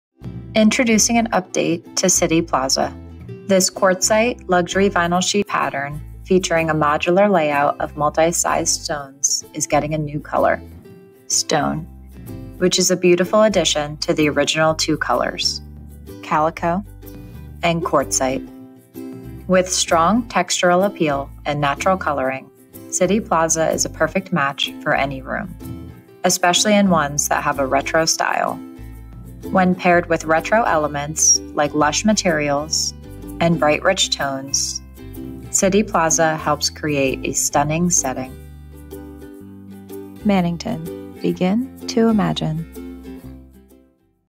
2020 Luxury Vinyl Sheet Intro: City Plaza
City Plaza is a quartzite pattern featuring a modular layout of multi-sized stones and a strong textural appeal. With its naturalistic coloring, City Plaza is a perfect match for any home. This year we're adding a new color, Stone, which will be available in stores in late March 2020.